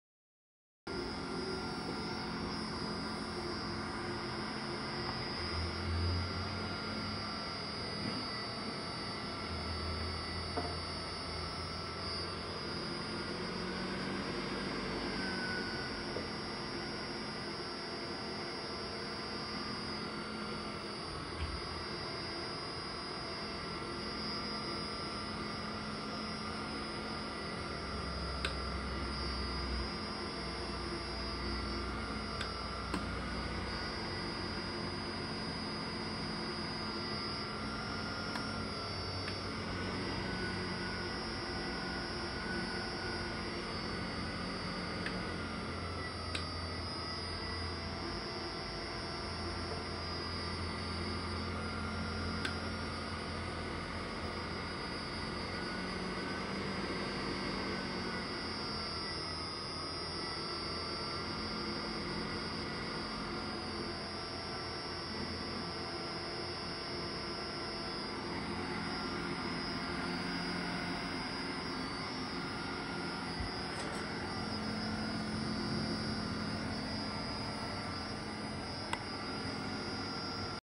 Ping Pong Levitation - CFW300 & Rockwell Sensor PID controlled
3 Setpoints or Potentiometer to set the height.
Parts used:

S7-1200 + AQ (TIA Portal V15 PID-Compact Controller used)
Rockwell 873P Ultrasonic Sensor 0-10V
CFW300 Frequenz Inverter
3 Phase Motor 0.5HP 1760 RPM ( heavily used)
Old Microwave Fan

The Frequenz Inverter operates between 70 and 100 HZ.